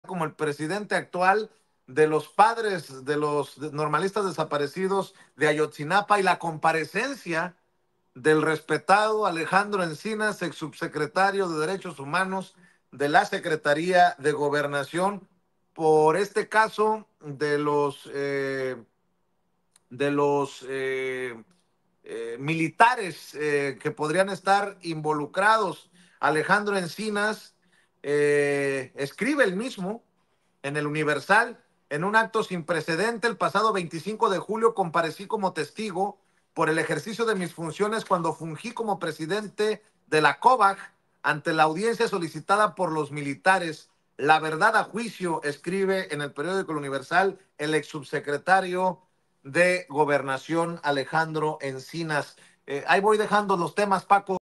¡SIN PRECEDENTES! ENCINAS COMPARECIÓ EN AUDIENCIA SOLICITADA POR M1L1T4R3S SOBRE CASO AYOTZINAPA
En el marco de la reunión de la virtual presidenta electa, Claudia Sheinbaum, con los padres de los normalistas desaparecidos de Ayotzinapa, Alejandro Encinas escribió en El Universal, en su columna “La verdad a juicio” que el pasado 25 de Julio, en un acto sin precedentes, compareció como testigo ante la audiencia solicitada por los m1l1t4r3s que podrían estar involucrados en la desaparición de los normalistas.